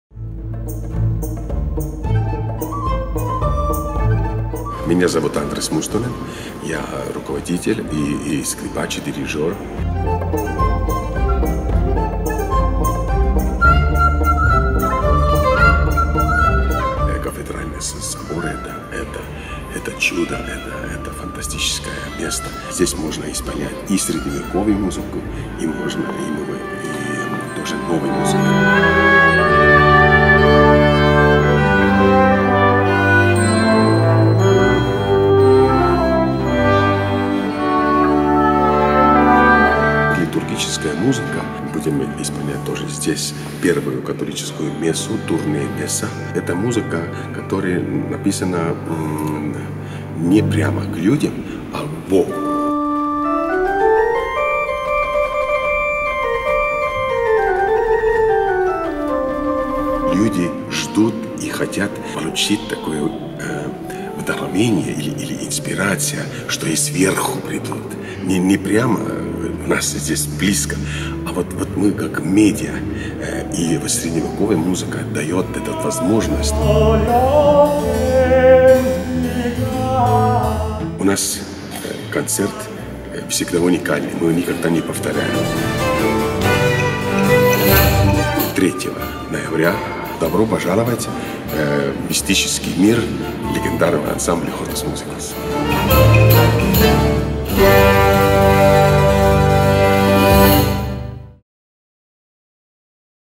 Hortus Musicus в Соборе на Малой грузинской | Анонс
Самая древний из известных нотных сборников был создан раньше Куликовской битвы. По нему до сих пор можно петь, и музыка эта дивно хороша, просто нужно уметь читать древние ноты. За эту сложную задачу взялся легендарный европейский коллектив – Hortus Musicus. Также в концерте прозвучат потрясающе красивые композиции Арво Пярта – композитора, соединившего в своем творчестве острую актуальность современности и прекрасное знание сакральных композиторских техник Средневековья.